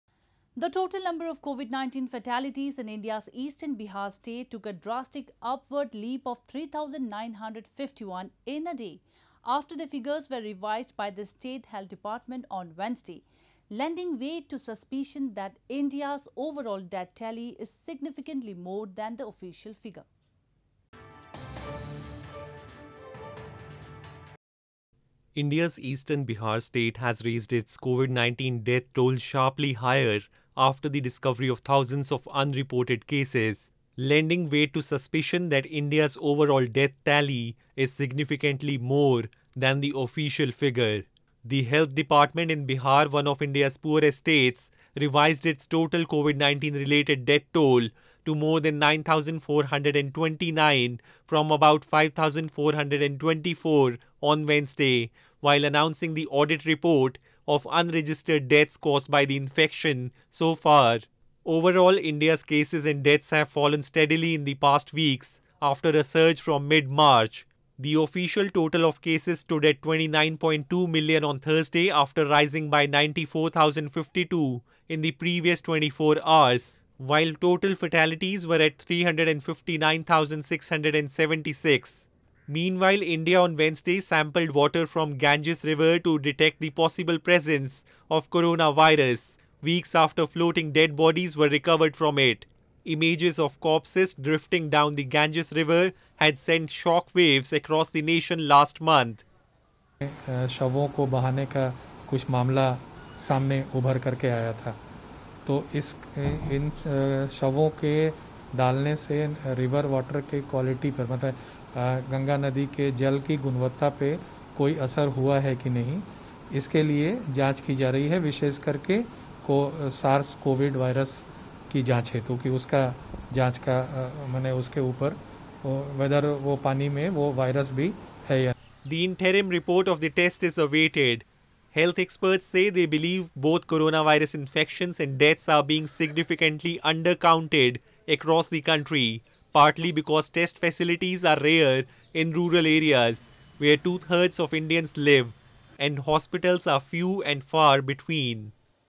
India's Bihar state sharply raises COVID-19 death toll prompting call for wide review
The total number of Covid-19 fatalities in India's eastern Bihar state took a drastic upward leap of 3,951 in a day after the figures were revised by the state health department on Wednesday, lending weight to suspicion that India's overall death tally is significantly more than the official figure. (KP)